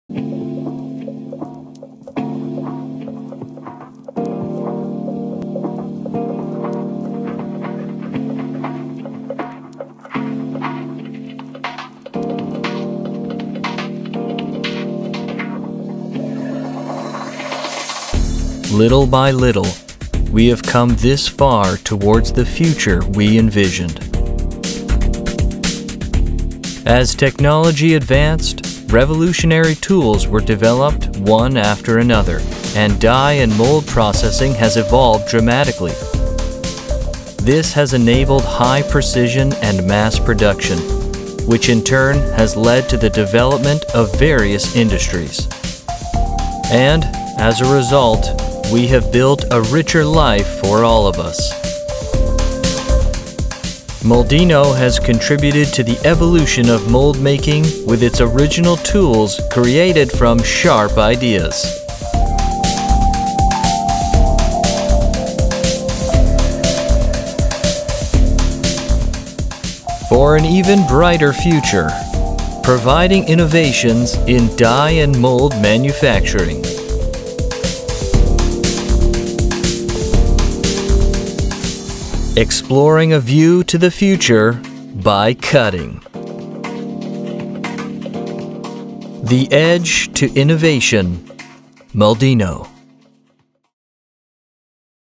Exploring a view to the future, by cutting
New technologies and concepts such as 5G, EV, and carbon neutrality are helping to move society forward. All of these trends demand higher precision in the manufacture of dies and molds. The ingenious tools made at MOLDINO are the result of listening to those working in these rapidly specializing fields.

Groundbreaking products so often rely on cutting-edge machining technologies to produce new types of dies and molds. In this sense, the sustainability of our future depends on the ability of companies like ours to develop innovative solutions to various challenges emerging on-site.

Let us enjoy cutting innovation with you together.

MOLDINO Tool search/CAD/CAM Support data pack/NC program support software